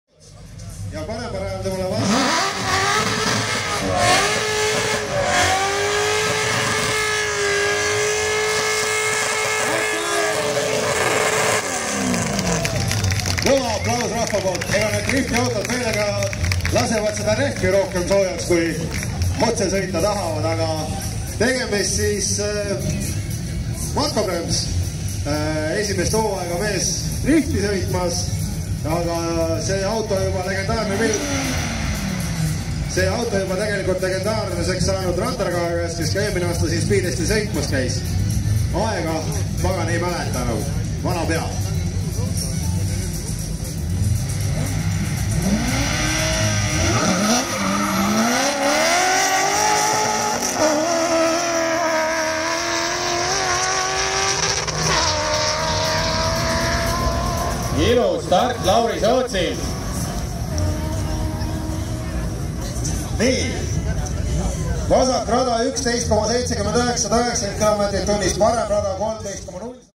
BMW E30 325ix Turbo vs BMW 320i E30 4.4 1/4 mile drag race
Lauri Soots 1987 BMW E30 325ix xDrive 2.5 I6 Turbo M20B25 vs Marco Prems 365HP 1990 BMW 320i E30 4.4 V8 M62B44 'ABGDriftTeam' 1/4 mile drag race @ Speedest Glassdrive Racing 2016, Pärnu airfield, Estonia.